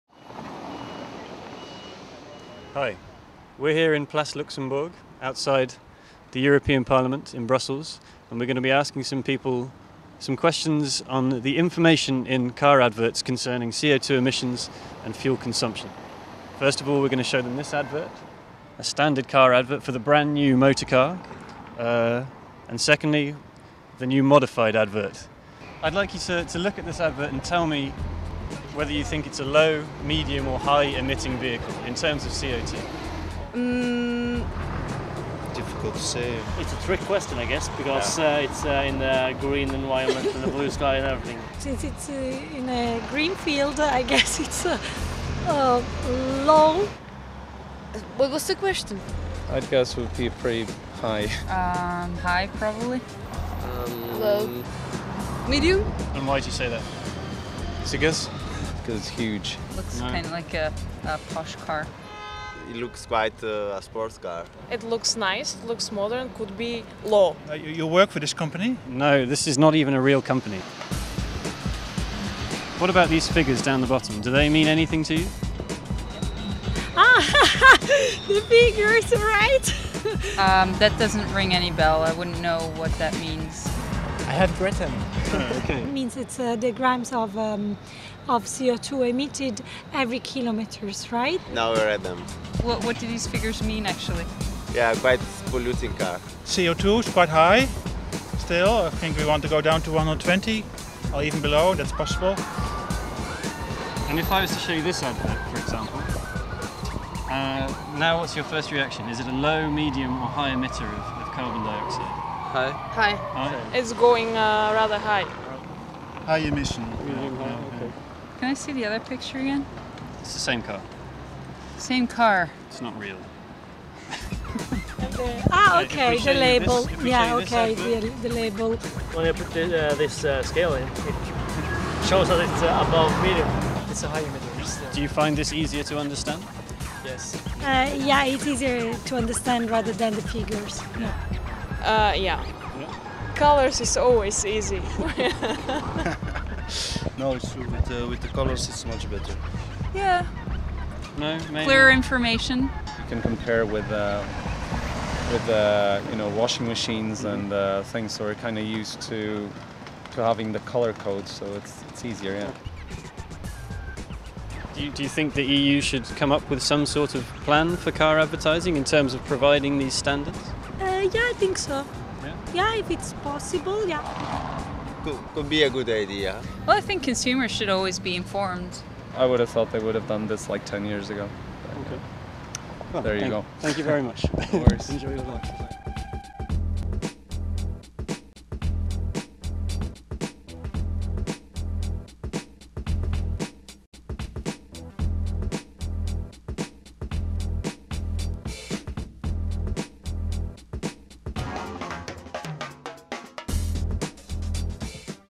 More colours in car adverts?
On the streets just outside the EU Parliament in Brussels we've asked people about the information in car adverts, showing two versions of our imaginary MOTOKA car advert.

In the first ad the info is very similar to the way it is currently displayed on car ads. In the second ad the info is more prominently displayed with a colour code label.

Which one is more clear? Do you understand the figures?